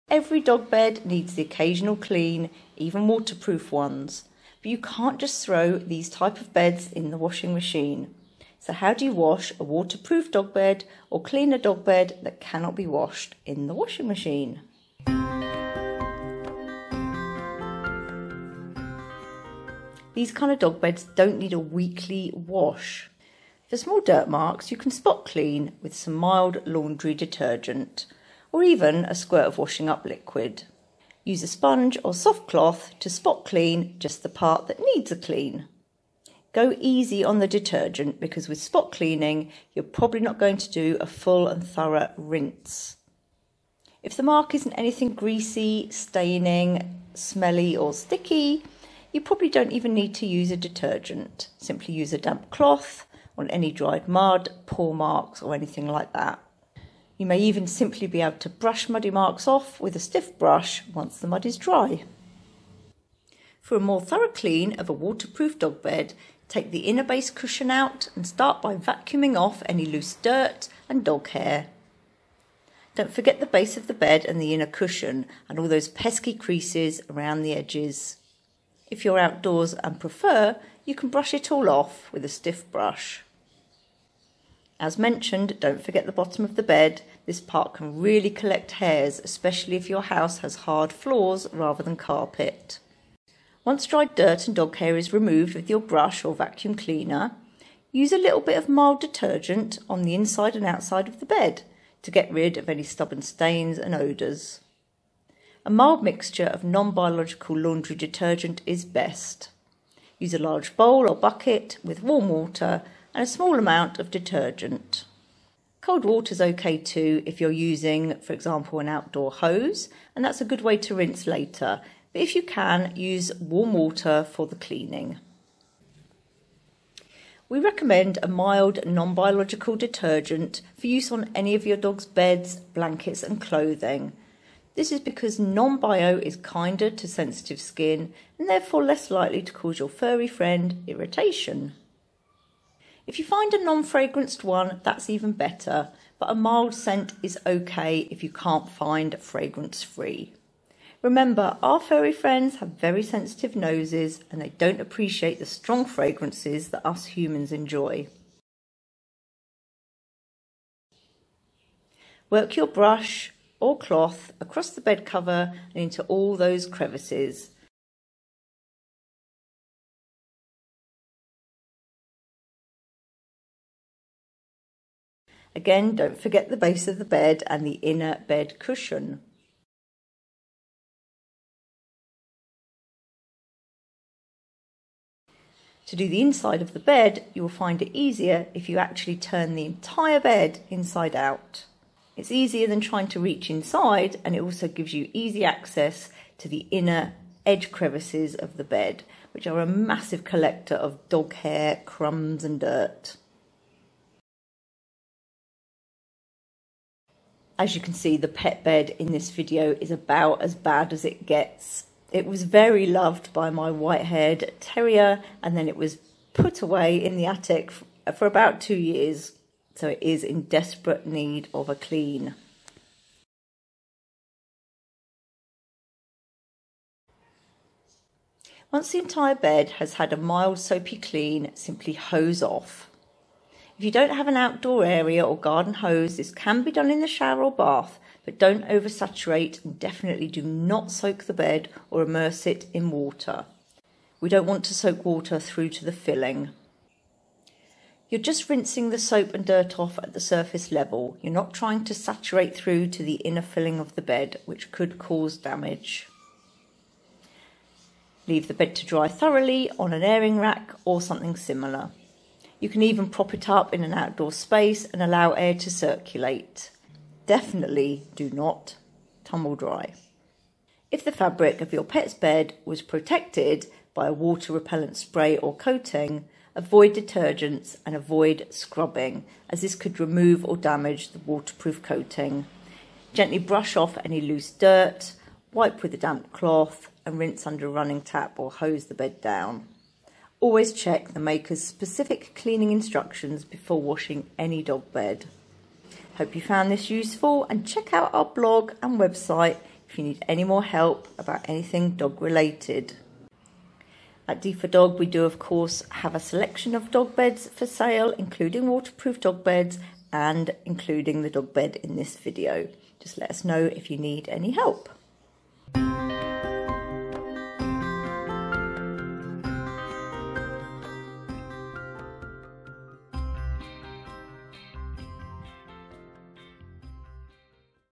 How to Clean a Waterproof Dog Bed
Waterproof dog beds are easy to maintain and keep clean. The waterproof fabric won't take or hold dirt and odours as readily as softer more traditional dog bed fabrics, so a waterproof bed tends to smell less and need less washing and cleaning. General dirt and dog hair sticks less to the durable waterproof fabric, which means less cleaning, washing and general maintenance. Great for a mucky pup or all dogs.

Every dog bed needs the occasional clean, even waterproof ones. But you can't just throw these in the washing machine. So how do you wash a waterproof pet bed?

In this video we show you how to spot clean your waterproof dog bed and how to do a more thorough wash as well.